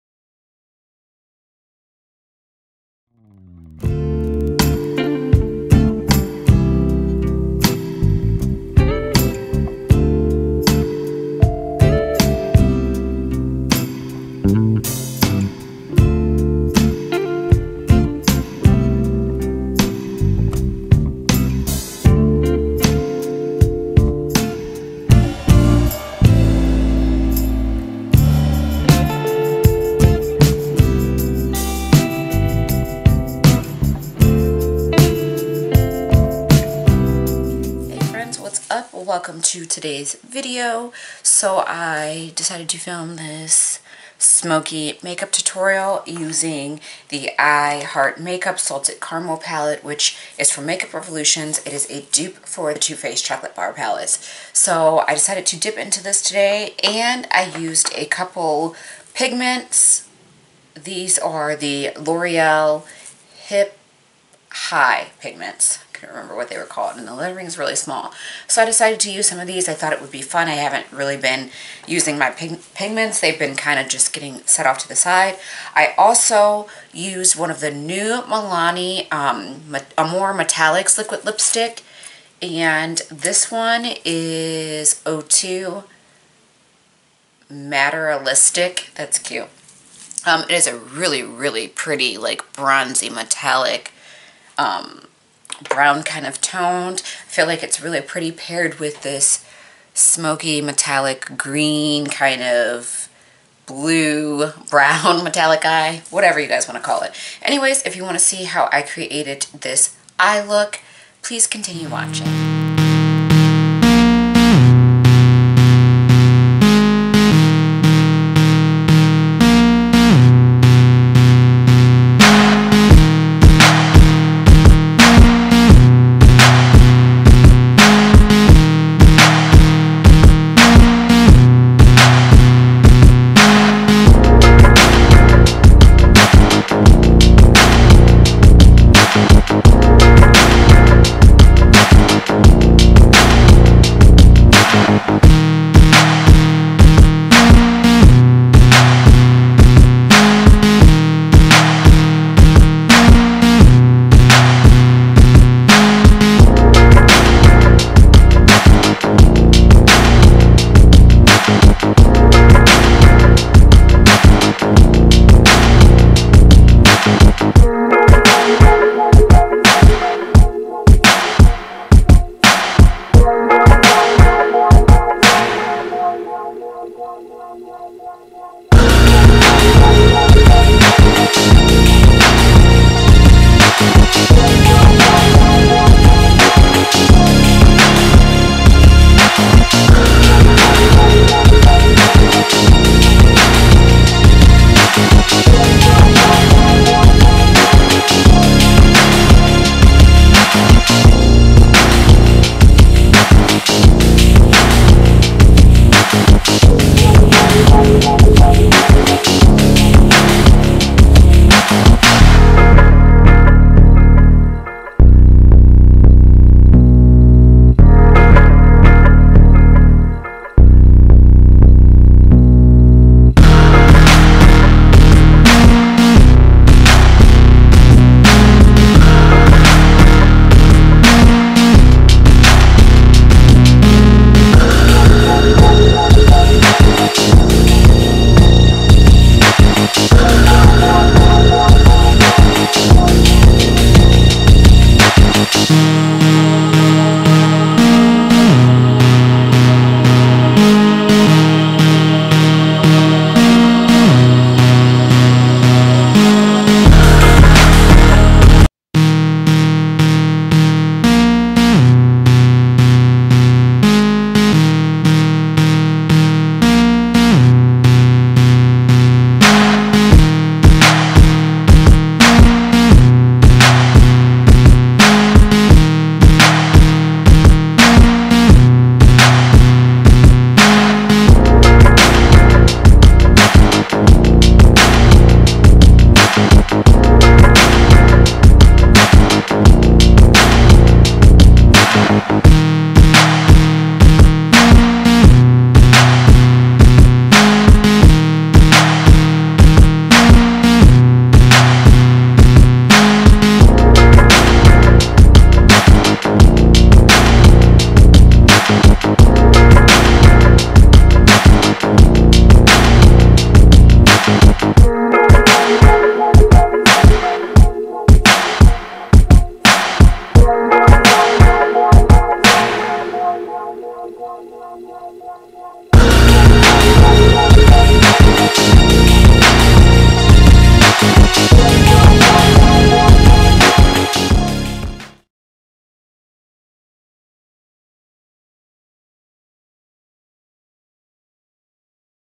MAKEUP TUTORIAL: Earthy Smokey eye & Bronzed Metallic Lip!
Hey friends another awesome tutorial here for you! Don't forget Once I hit 500 subscribers I will do a huge giveaway so please spread the word, this one is going to be big!I love you guys.